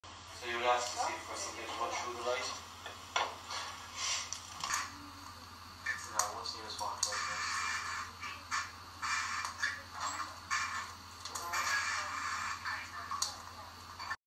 See what we Captured at leap castle !!!
Captured Light anomalies at one of Irelands Haunted Castles Leap Castle. This was captured on the ground floor one in the centre of the room where many EVP and shadows have been seen. The other over by the walkway. Often low energies have been felt here….